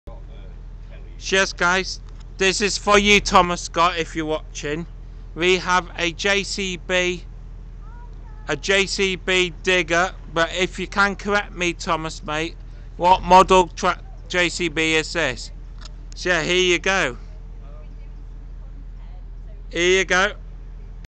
VIP TRIP TO BIKER GROUP 2022
so yes here I'm standing outside of a jcb digger with a bucket this is recently new to biker group there's 1 in Scotland and this one is the new one to biker group this is for you Thomas Scott so Thomas mate if you can tell me the model that be great